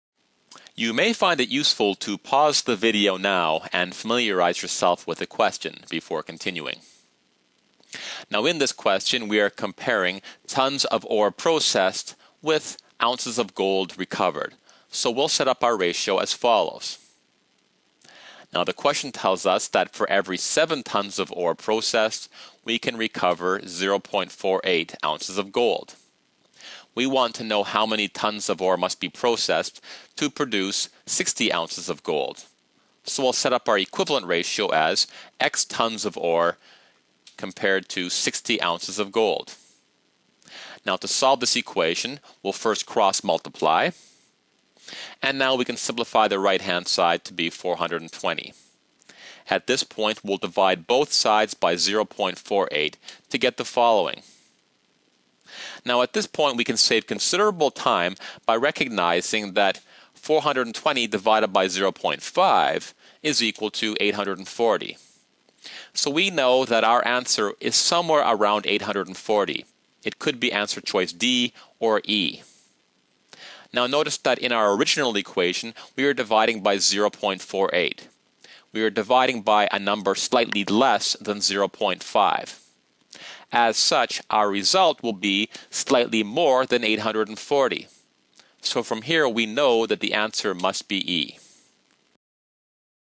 GMAT Math Tips: Ratio Shortcut
Brushing up on math for the GMAT? Watch this short video to learn a useful math shortcut using ratios!

00:00 - Intro to the video
00:10 - What we're comparing
00:20 - Setting up the ratios
00:40 - Solving